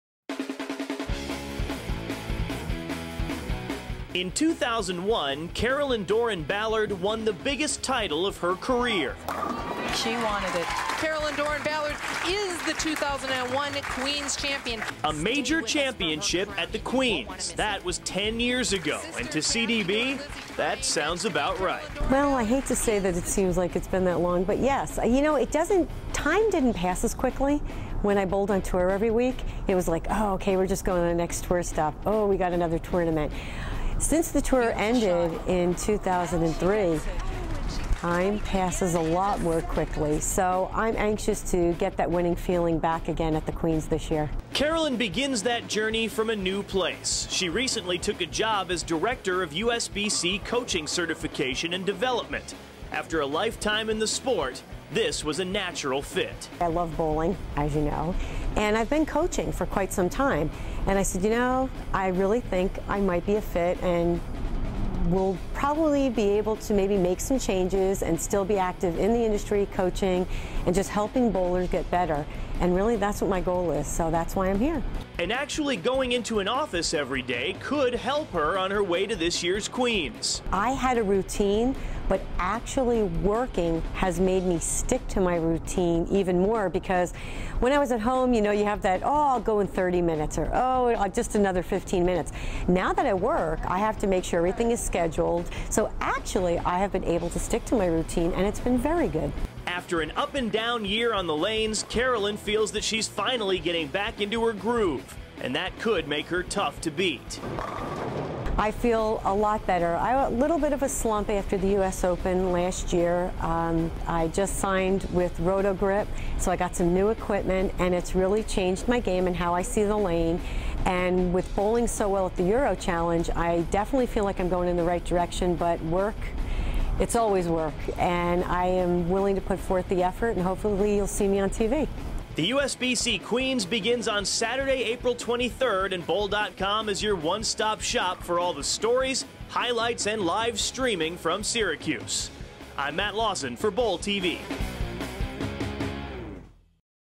Queens Preview: Carolyn Dorin-Ballard
Carolyn Dorin-Ballard is the new USBC Director of Coaching Certification and Development.  BowlTV talks to her about her new job and her chances at this year's USBC Queens tournament.